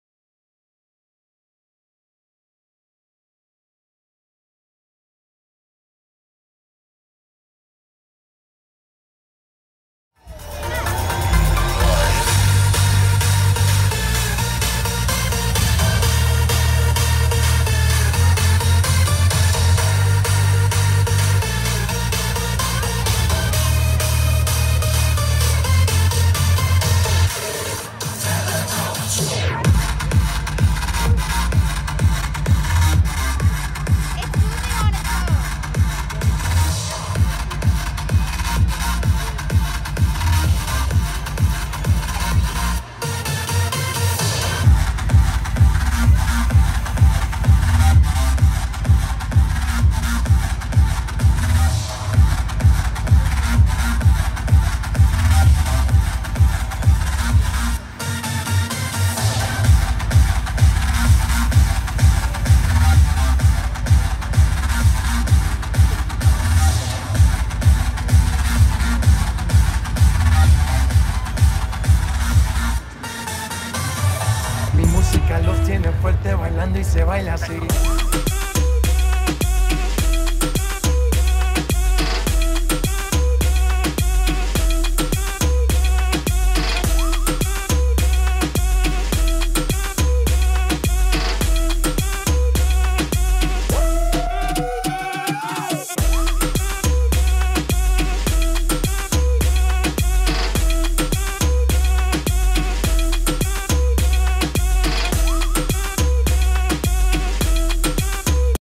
Victoria Circus Light Show
Sensational Murcia Productions presents Victoria Circus Thrill Show. Featuring the best L.E.D. stage lighting and effects of any outdoor show in the fair industry. Night show performances of our Circus and Thrill acts are enhanced with moving head washes, gobos, vertical foggers and more!